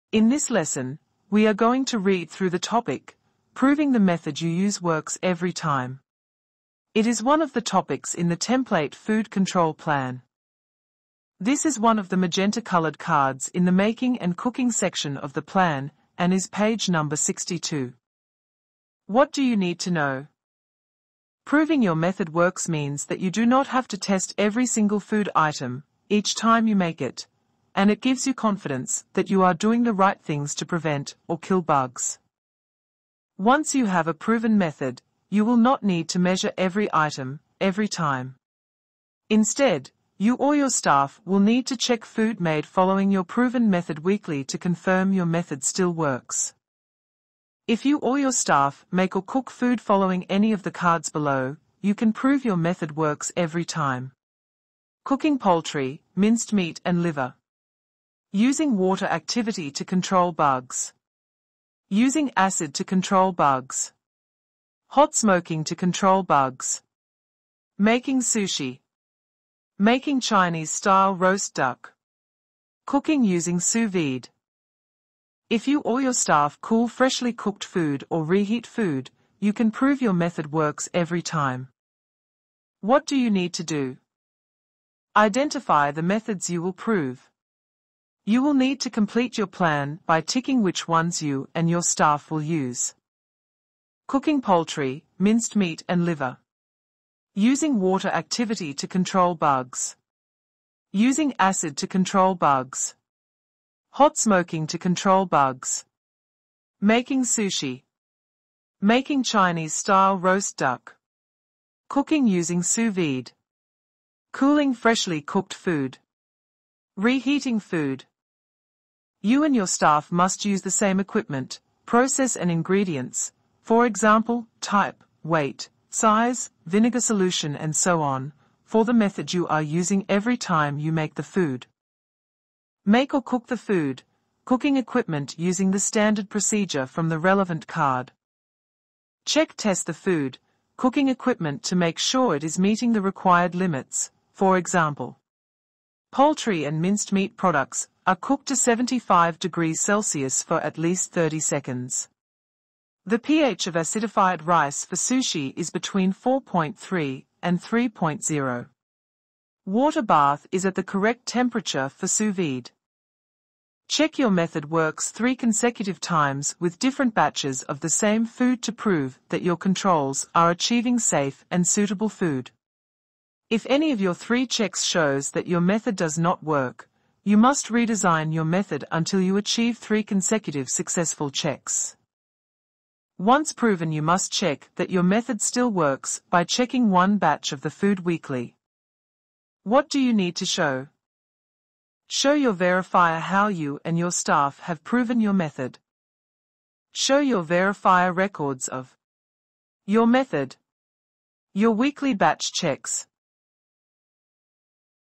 Proving the method you use works every time
In this topic from the template FCP you will learn how you can use proven methods of ensuring your food is safe and suitable every time you cook it and without the need to check every item of food every time.
You still need to undertake weekly checks to ensure continued validation of the method you are using.  Also the topic refers to several other topics in the plan which include methodology for proving safe cooking.